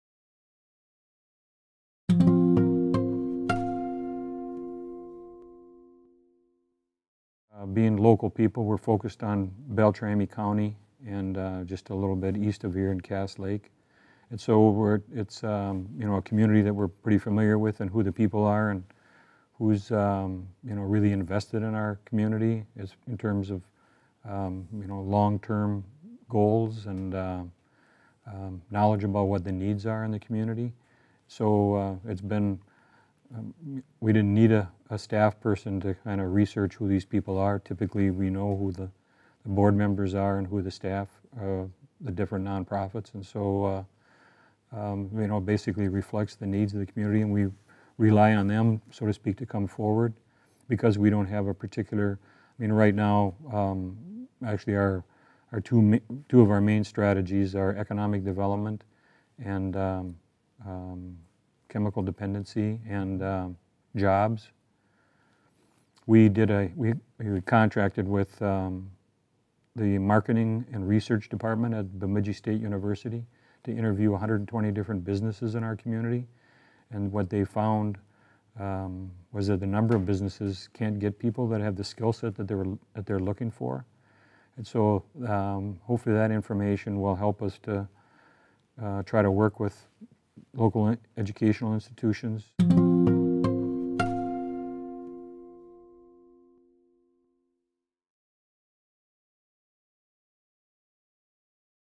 Preview: George W. Neilson Foundation - Paul Welle
Paul Welle, Chairman of the George W. Neilson Foundation, discusses how the foundation supports local nonprofits to meet the needs of their communities.